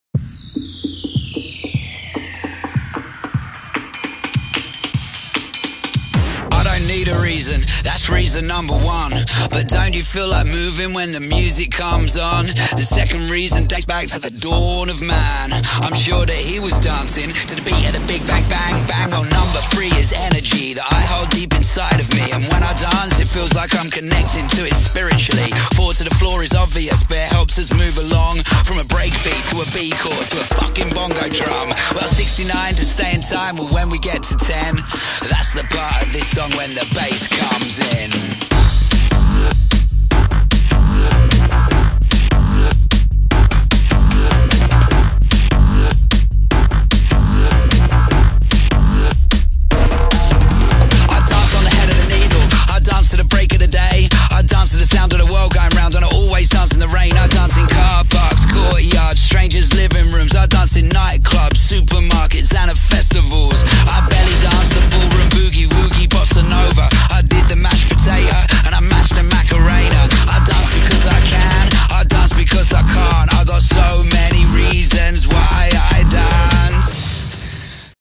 Palais De Phonix - Solfest Festival 2022
Unfortunate i couldn't make the whole weekend but here's my Friday Palais de Phonix @ Solfest  video, Please View Share & Tag Away